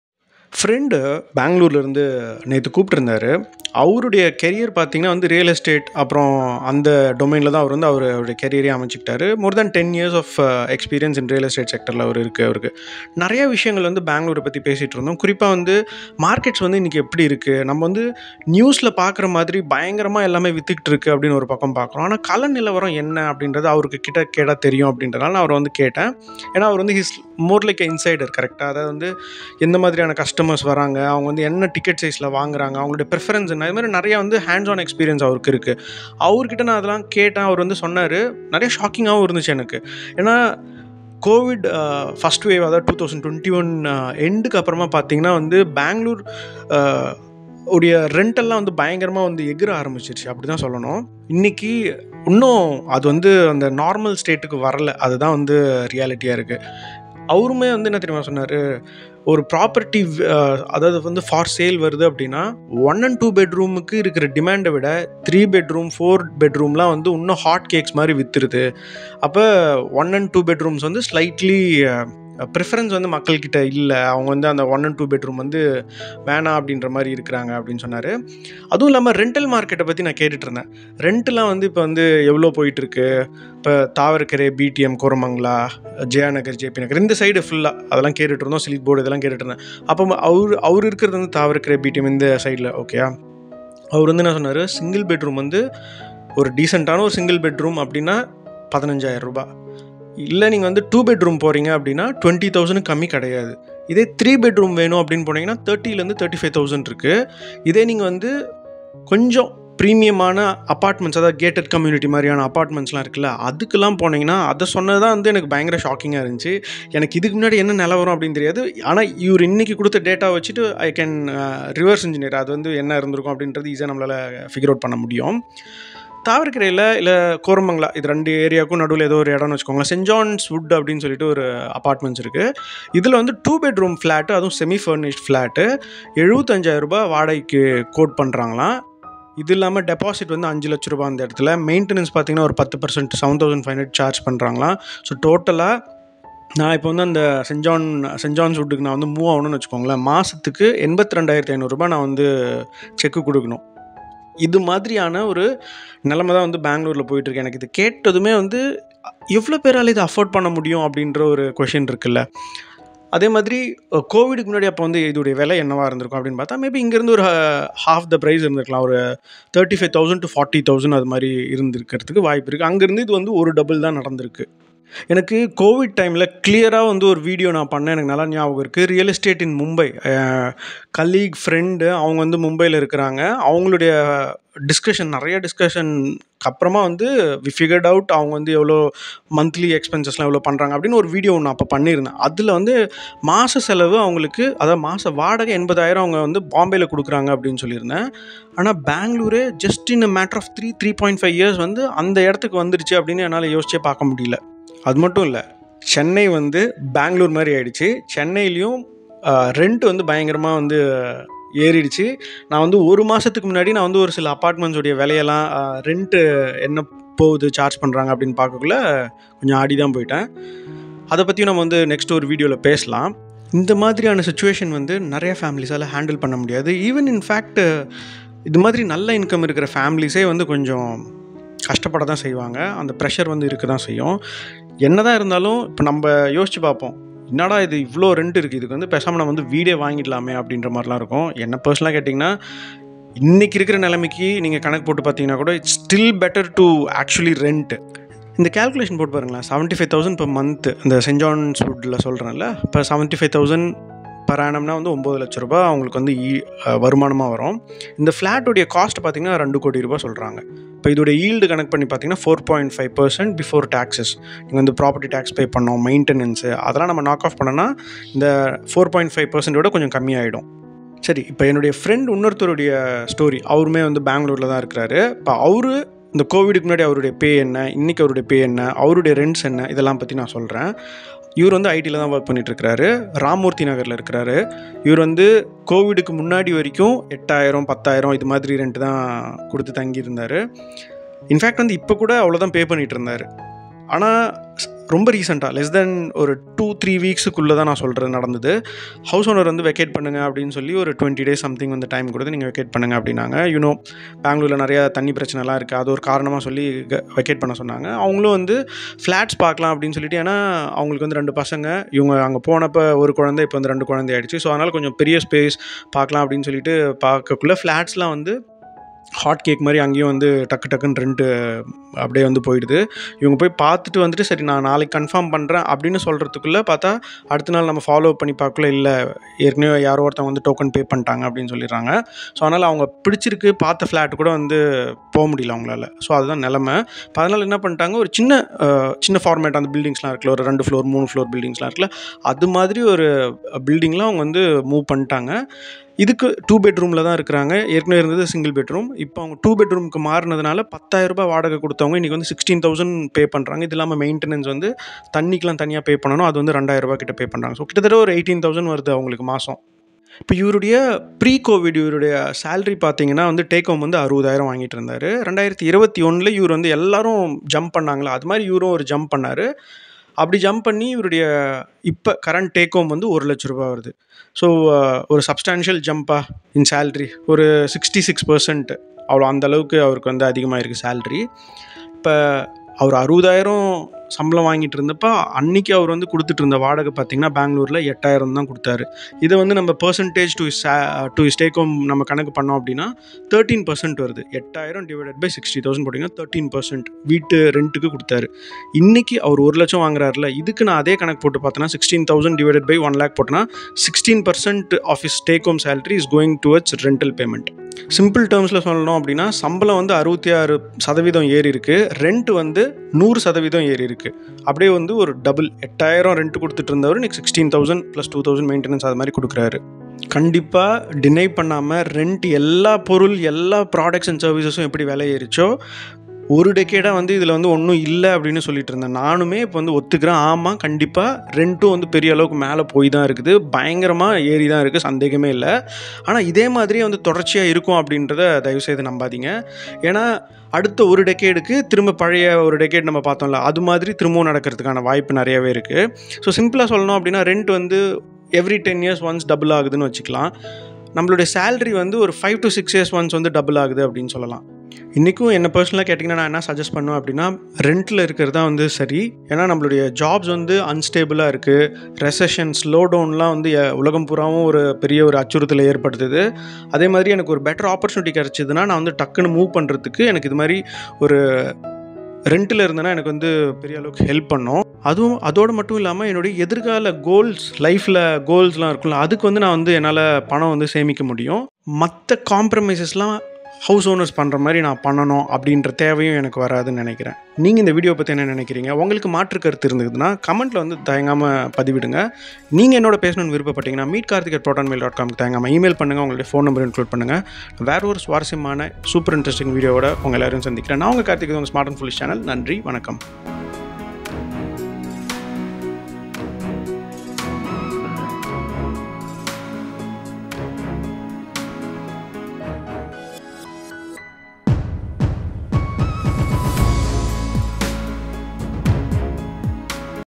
Shocking: என்னது 2 BHK வாடகை Rs.75, 000?! Sky High Real Estate Rental Market | Bangalore Rent Crisis
Why is Bangalore Housing Market Super Hot?